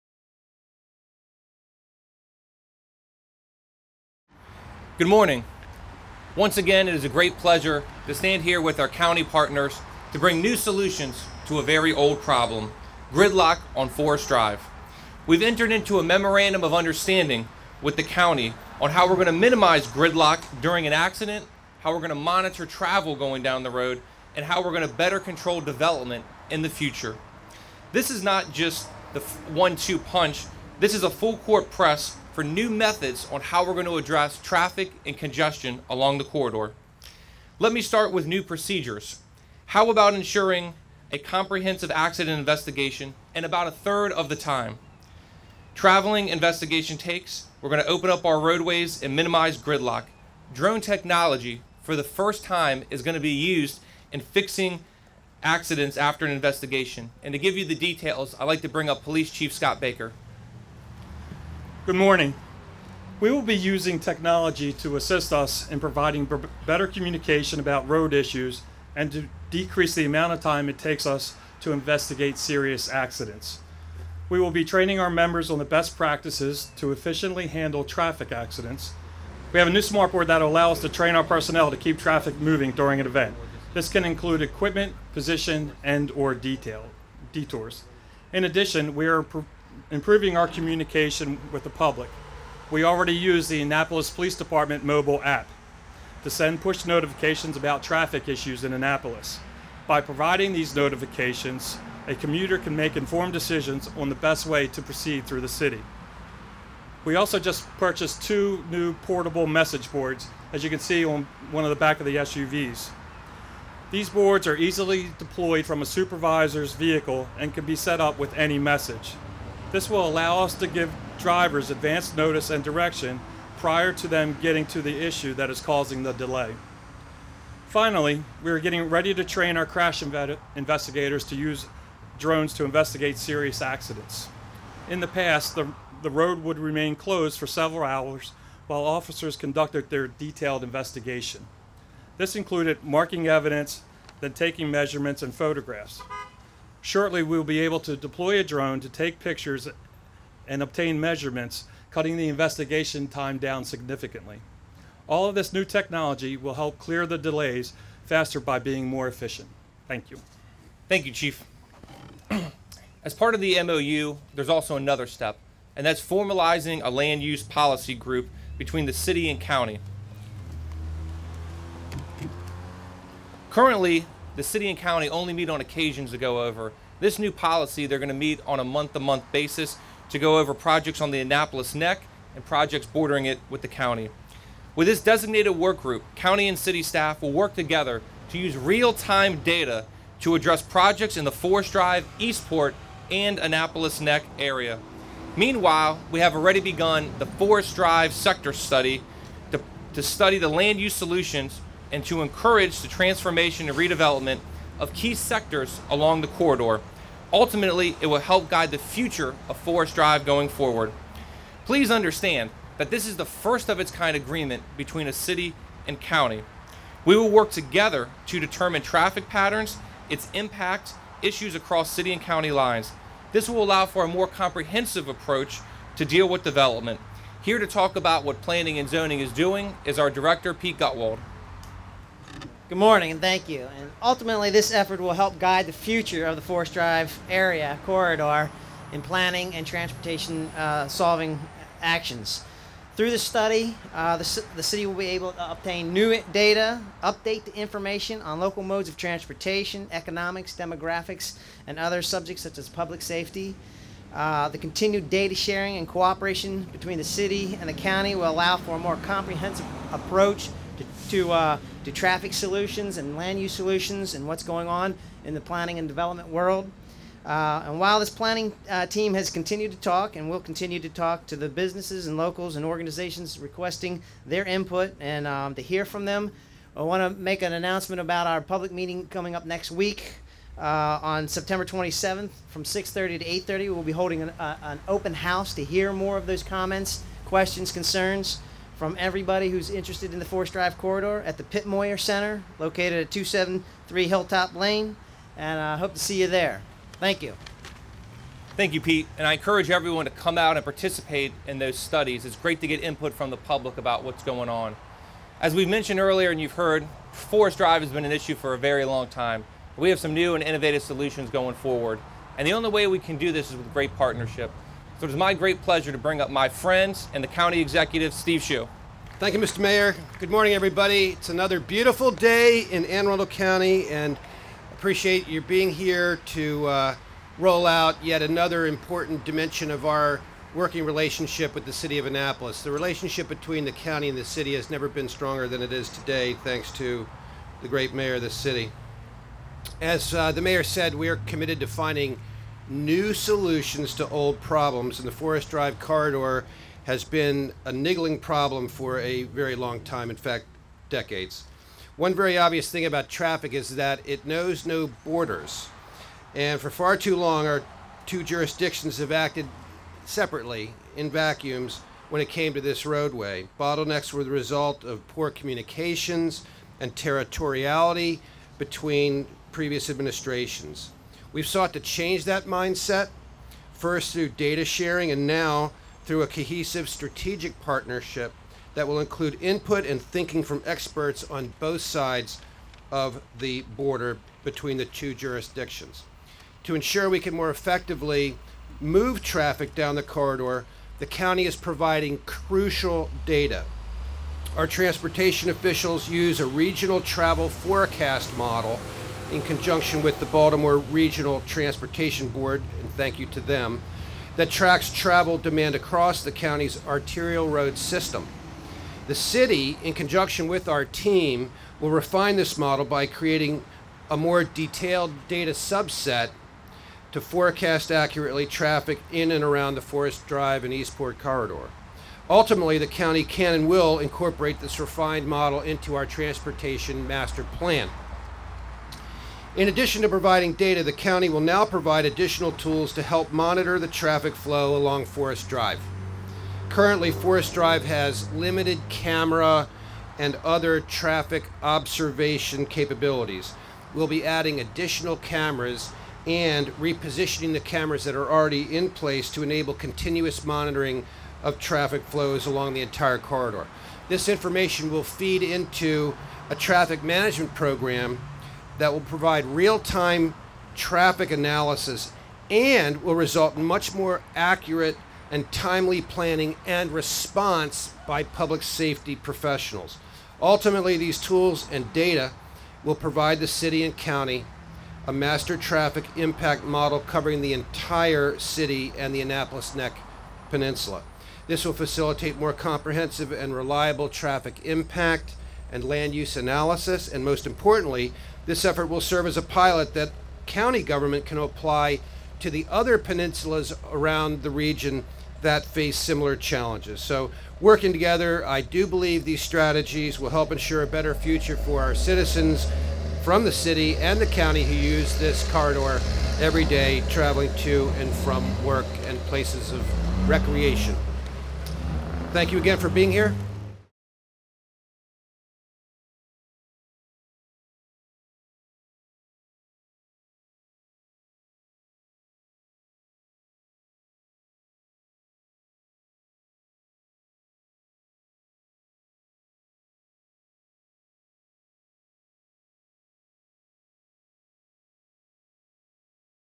Forest Drive Corridor September 21, 2017
Annapolis Mayor Mike Pantelides and Anne Arundel County Executive Steve Schuh announce initiatives to help traffic issues on Forest Drive in Annapolis.